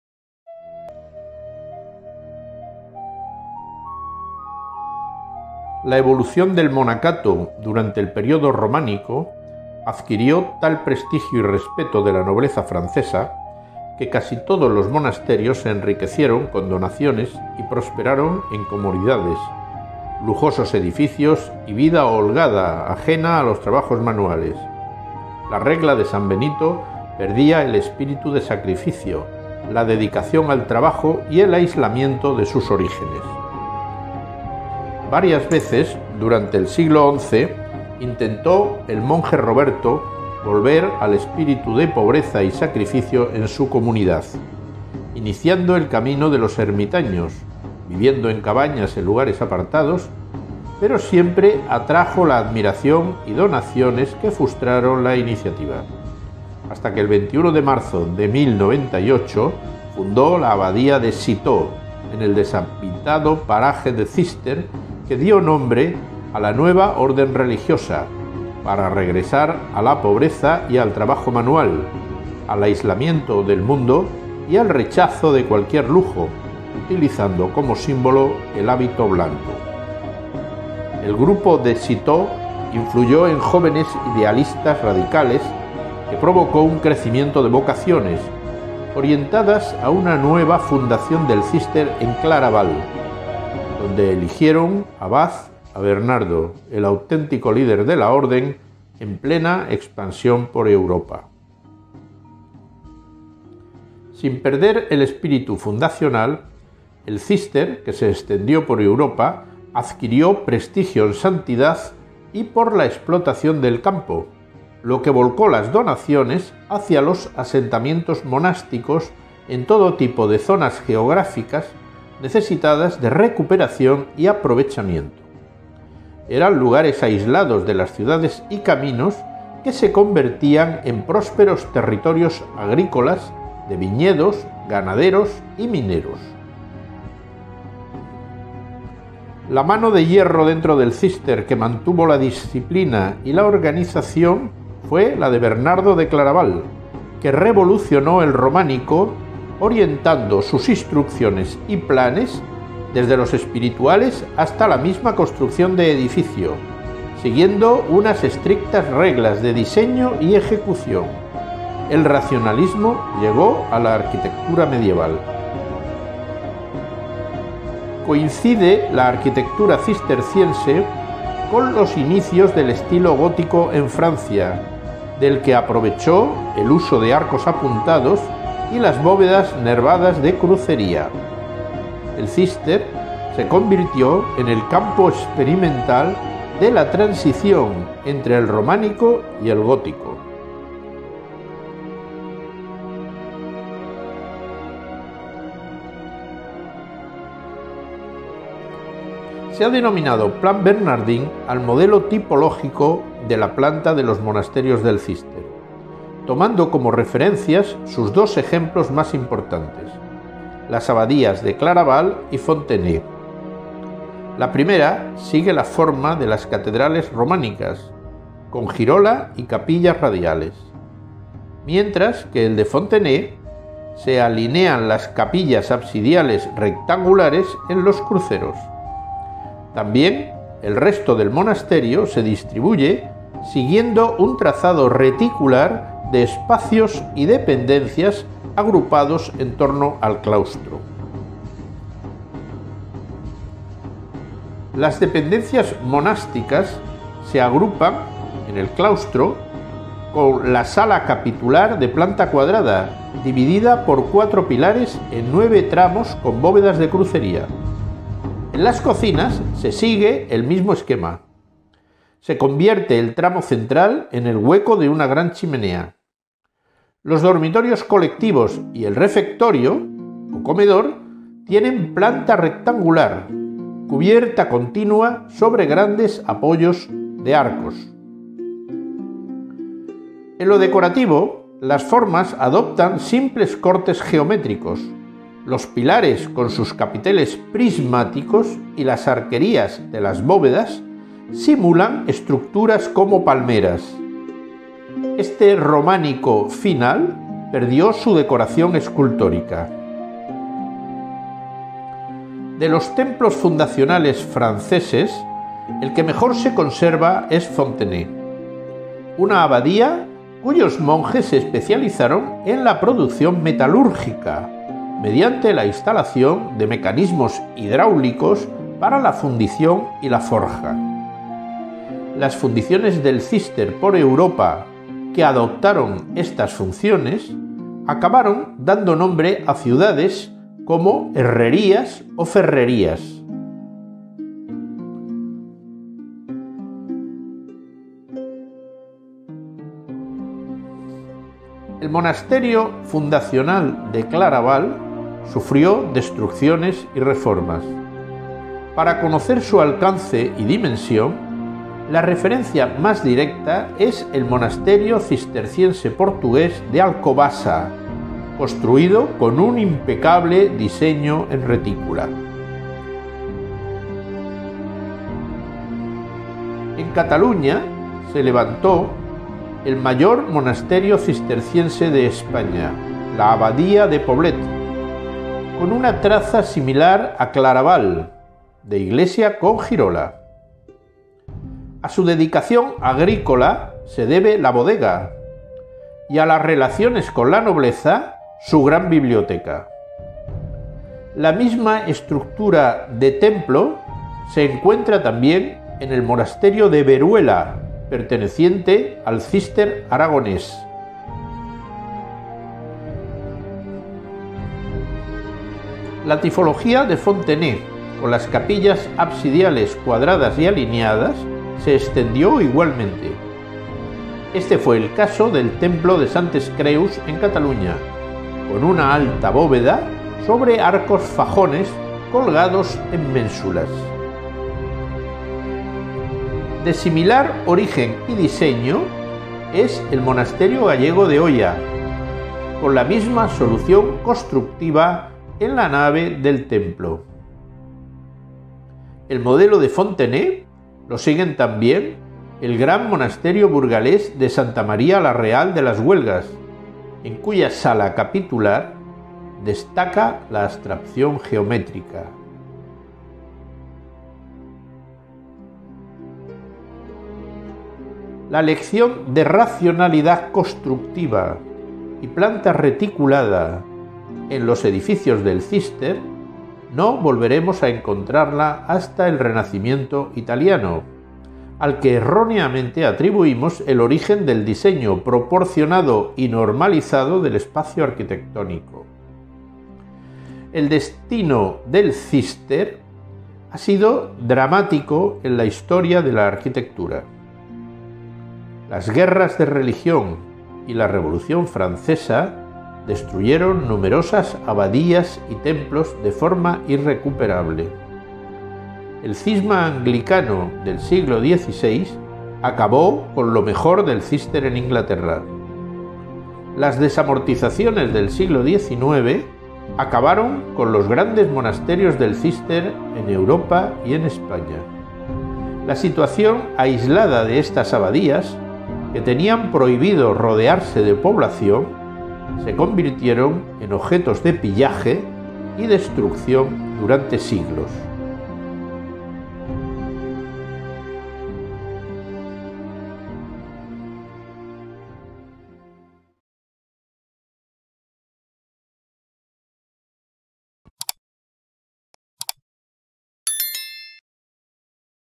EL CISTER. LA REVOLUCIÓN ROMÁNICA
SERIE MEMORY Nº 15. La renovación del monacato medieval por la orden del Cister, generó un drástico cambio antidecorativo en la arquitectura románica.